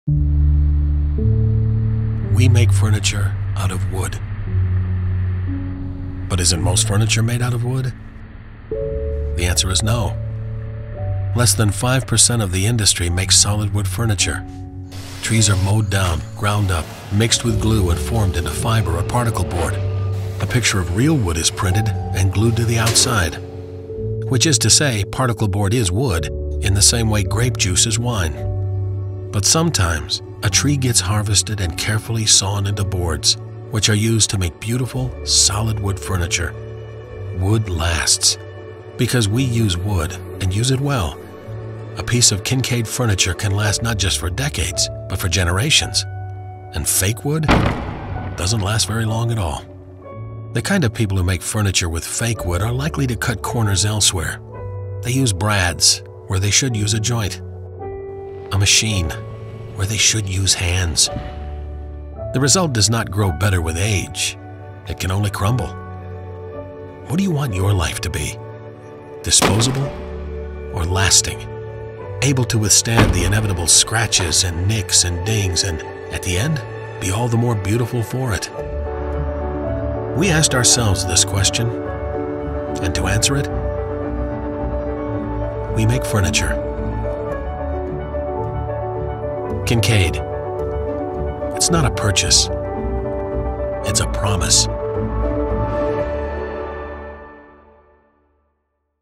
Kincaid Promise Video
Learn what sets Kincaid furniture apart from others. It's not a purchase ... It's a promise.